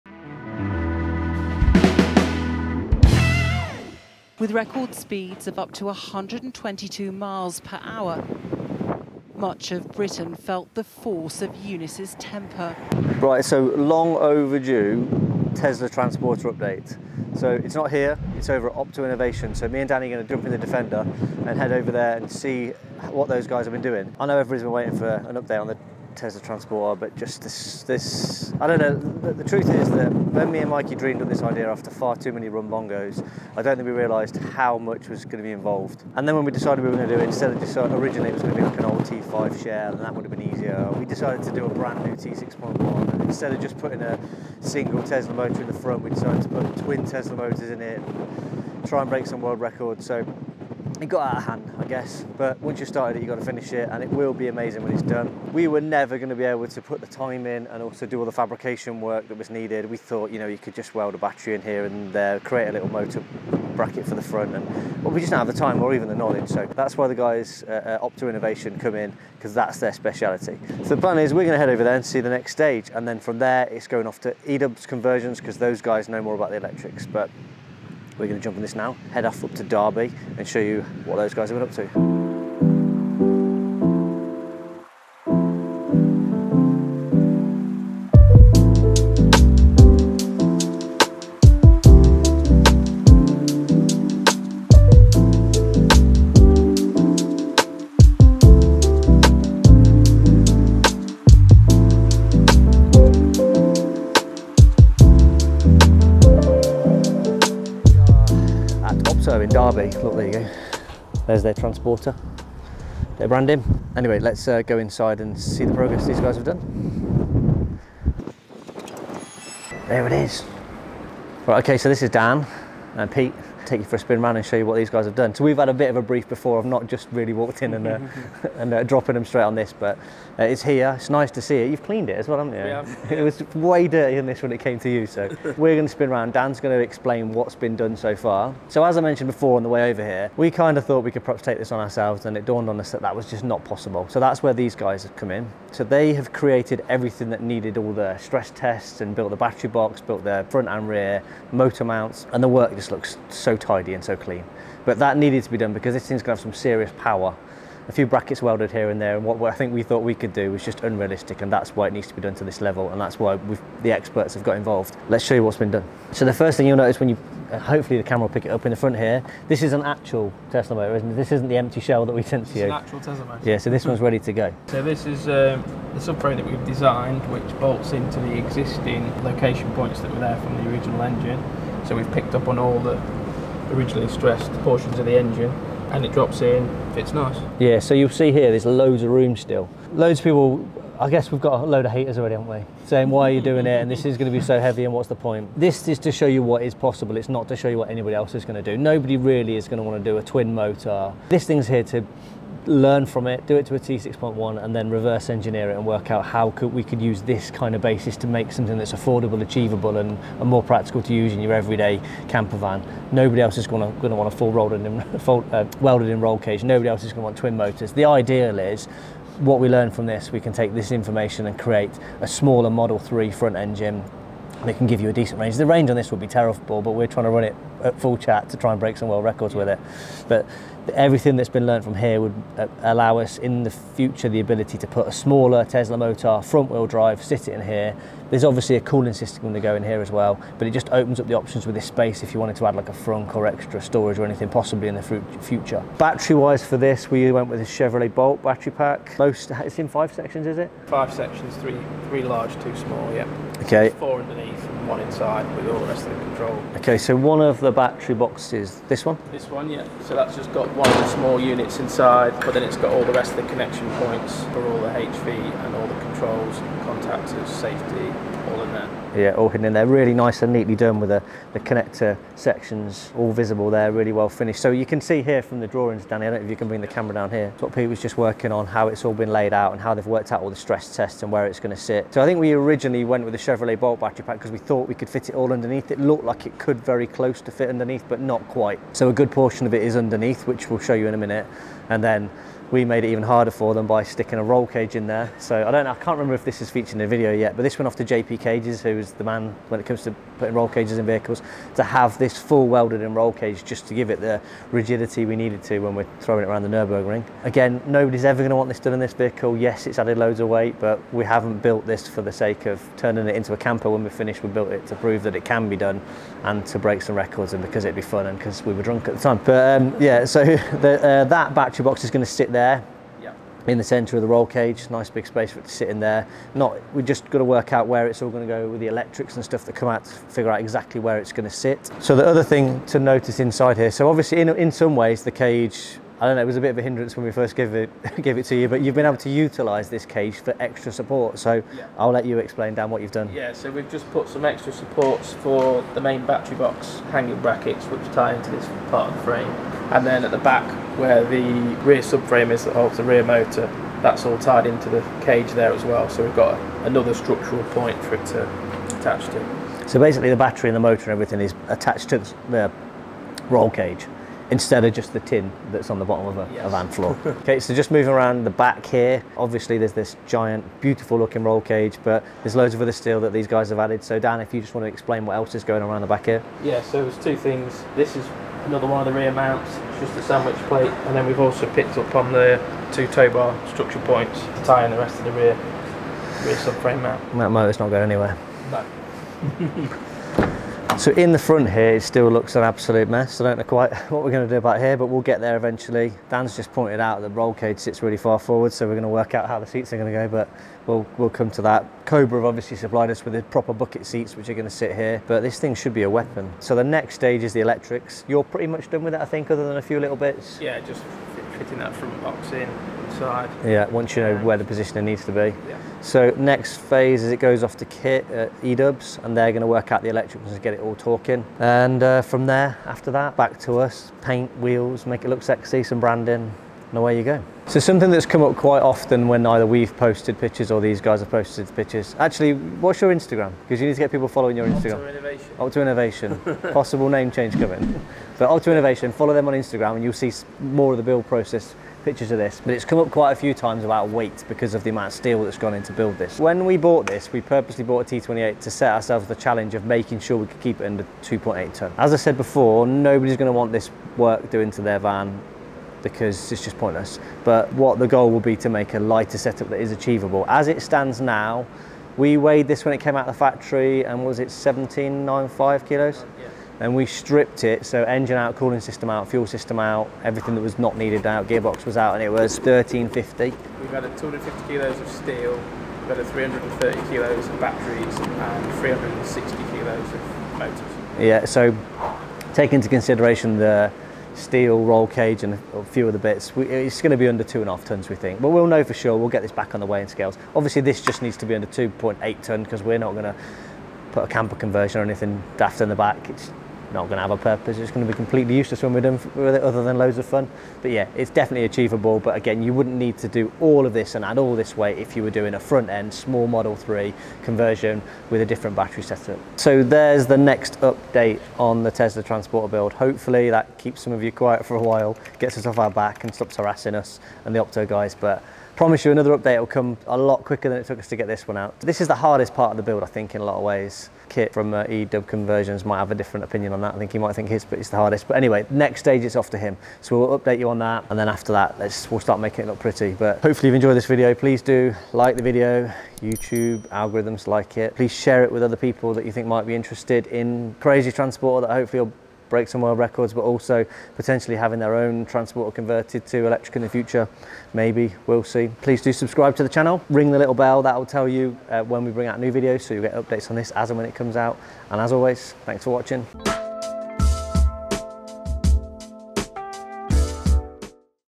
Tesla powered VW Transporter T6.1 updated. batteries, twin Tesla motors and roll cage in. Episode 5
Sorry it's taken so long to get this update out. The next stage will happen a lot quicker.

We head over to the guys at Opto to see the fabrication work on the front and rear motor mounts, plus the battery boxes. We've never shown you the amazing roll cage by JP Cages before so you'll get to see that too.